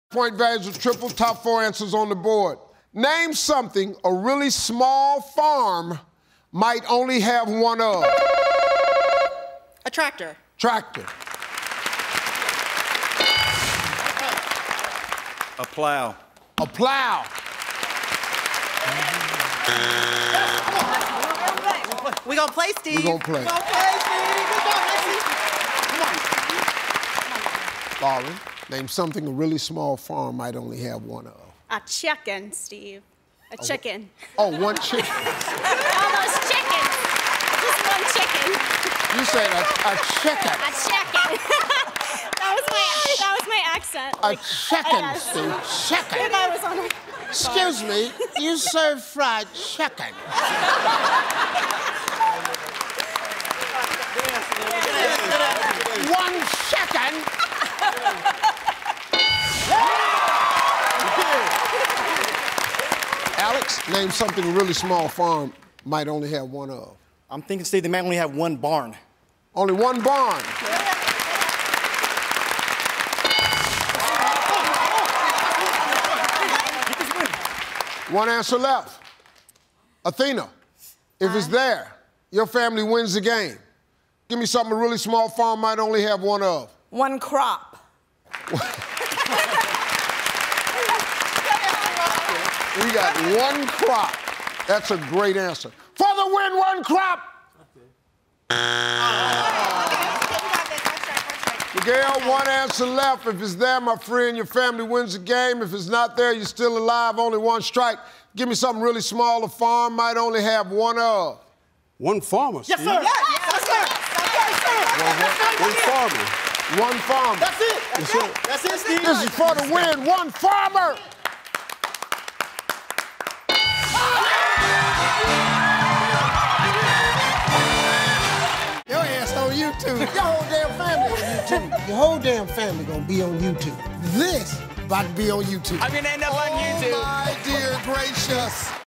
Small farms have only one what??
Name something a really small farm might only have one of.